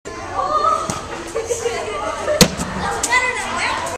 Cornhole Practice
Hilliard Elementary students practicing cornhole in Physical Education class.